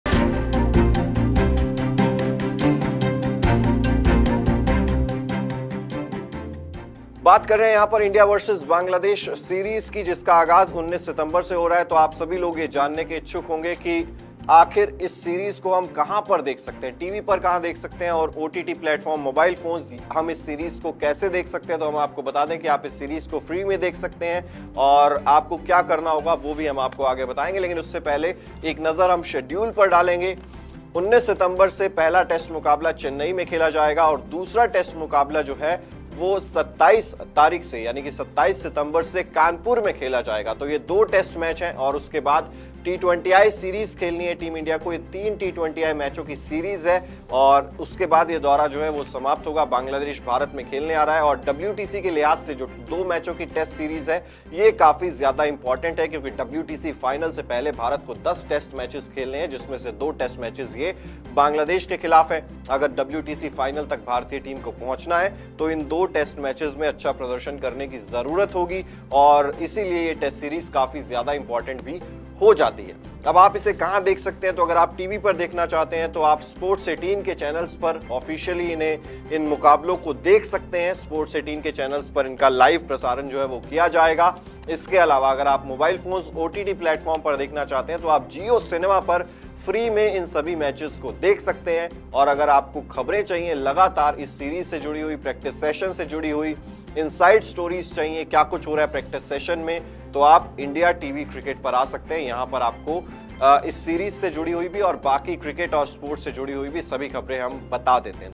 Where to Watch IND vs BAN Test Match: India vs Bangladesh Test Series Free Channel and Live
The excitement is about to begin between India and Bangladesh. The first match of the series is from September 19, which will be played in Chennai. After this, the second match is to be held in Kanpur. Not only this, after this a three-match T20 series will also be played. Meanwhile, now the series is near, so you should know how you can watch the matches of this series on your TV and mobile.

भारत और बांग्लादेश के बीच अब रोमांच शुरू होने वाला है। सीरीज का पहला मैच 19 सितंबर से है, जो चेन्नई में खेला जाएगा। इसके बाद दूसरा मुकाबल कानपुर में होना है। इतना ही नहीं, इसके बाद तीन मैचों की टी20 सीरीज भी खेली जाएगी। इस बीच अब सीरीज करीब है, ऐसे में आपको जानना चाहिए कि इस सीरीज के मैच आप अपने टीवी और मोबाइल पर कैसे देख सकते हैं।